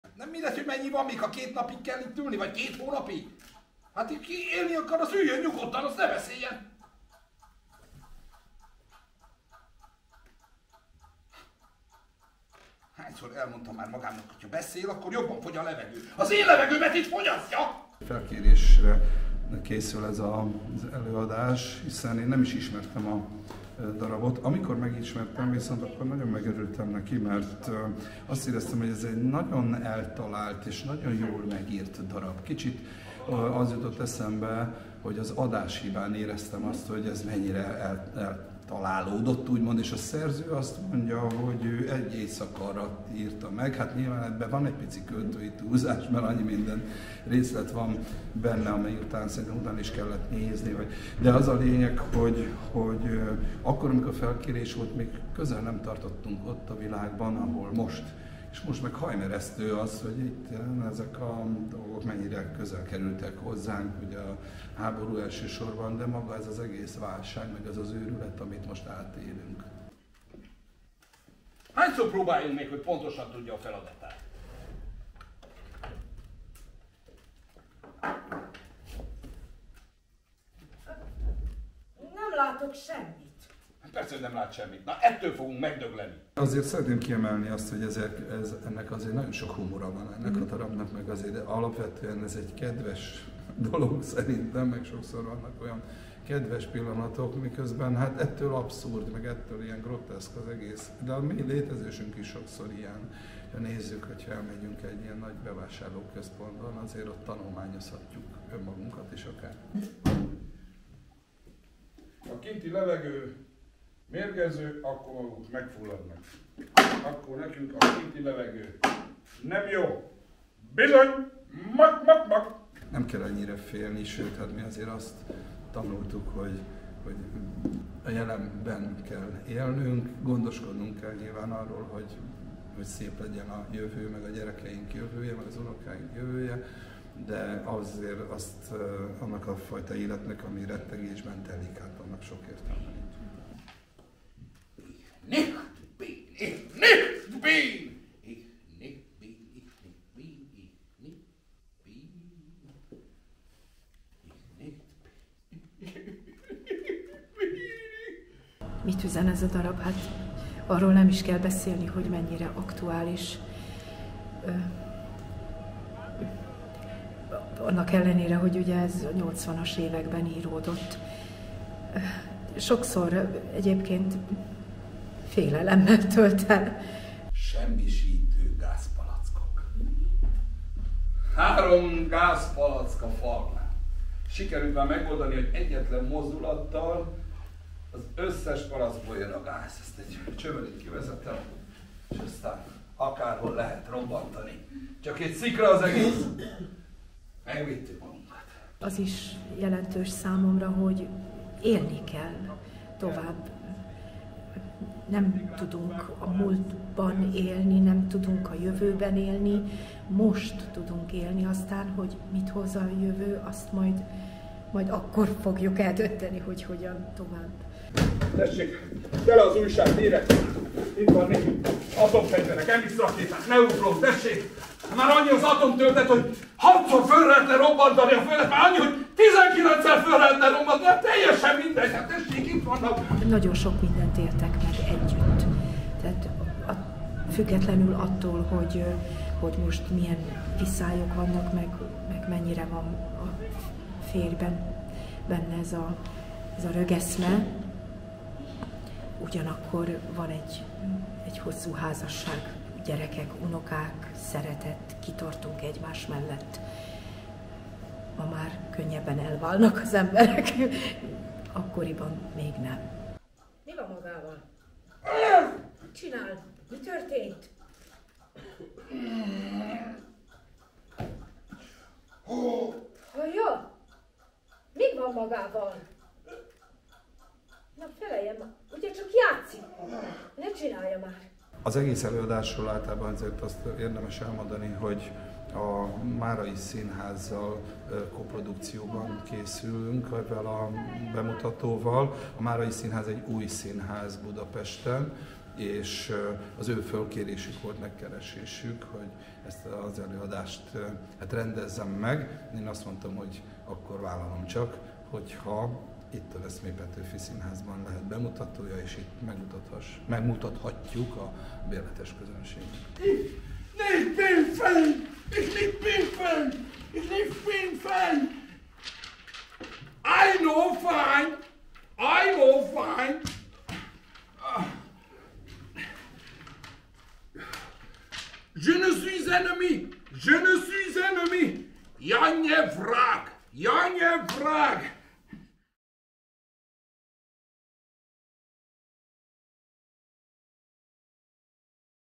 Nyolcvan vödör levegő a Latinovits-Bujtor Játékszínben
LEZSÁK SÁNDOR
NYOLCVAN VÖDÖR LEVEGŐ
tragikomédia
Rendező: OBERFRANK PÁL Jászai Mari-díjas, Érdemes művész

Mindenki fél valamitől vagy valakitől. Falakat építünk magunk köré és hisszük, hogy megvéd. Megvéd, de bezár... A Lezsák Sándor által megálmodott idős házaspárt a félelem vezérli, megépítenek egy bunkert, és készülnek. Félnek, hogy elkezdődik a rettegett állapot. De mi is, és hogyan? A darab fanyar humorával erre is választ ad és megmutatja, hogy miért fontos hinni valamiben...